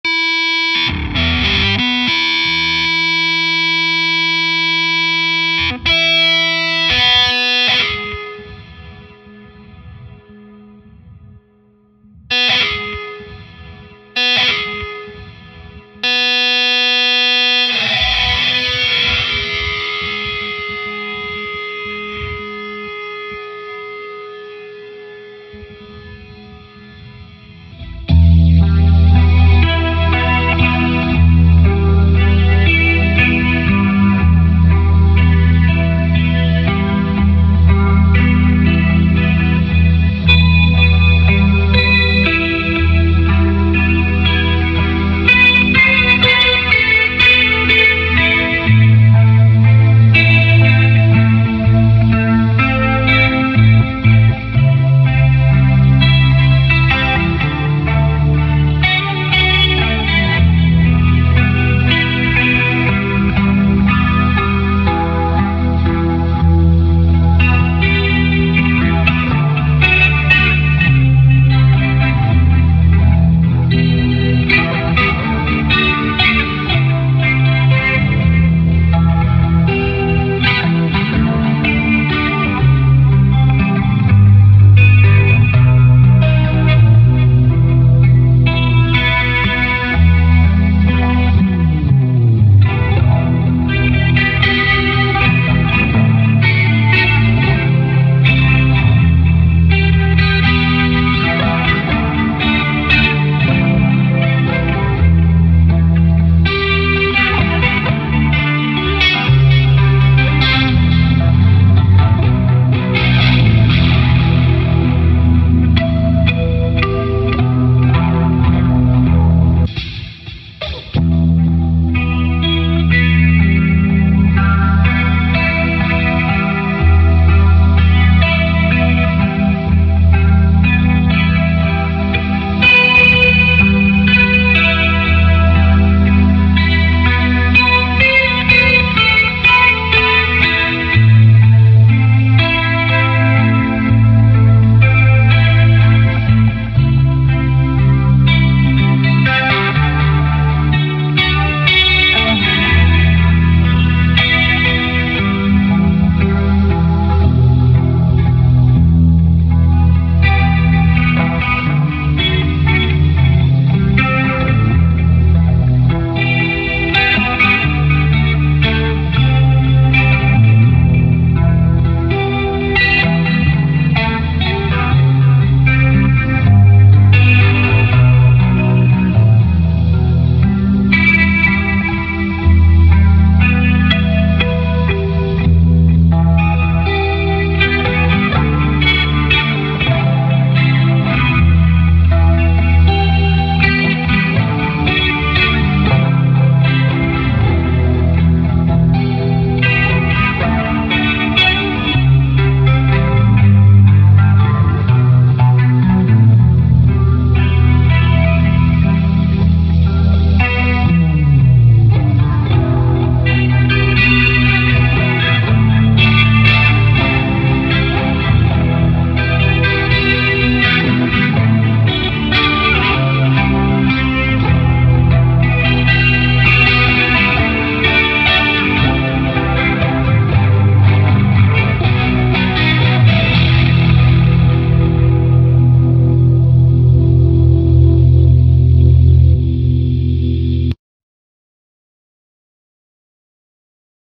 OxyDac - Broken string
The guitar string broke and got between my fingers, it totally confused me for a moment ( tilt...wtf happened? )
I somehow managed to tie the broken string, so later I tested it a little...